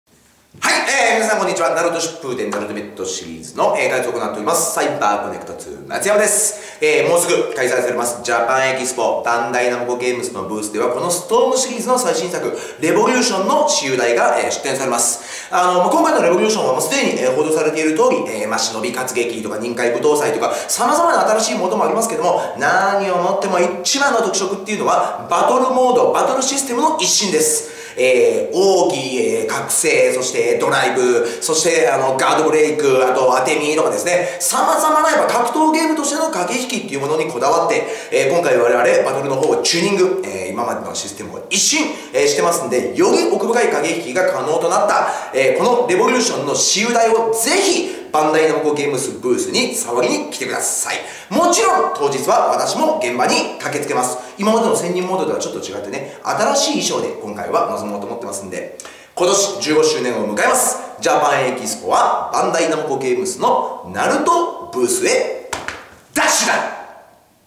Naruto SUNS Revolution @ Japan Expo 2014 - Matsuyama-san has a message for you (English)
Hiroshi Matsuyama, CyberConnect2 super star, will be at Japan Expo 2014! Come find out straight from the main man why the next installment in the Naruto Shippuden: Ultimate Ninja Storm series is a true revolution ;)

BANDAI NAMCO Games Europe will be presenting its full line-up during the Japan Expo at Booth BF76 Hall 6. Get ready to discover fresh information on the Naruto game and many more surprises!

Naruto Shippuden Ultimate Ninja Storm Revolution will be available September 2014 for the EMEA territories on PS3, PC and Xbox 360.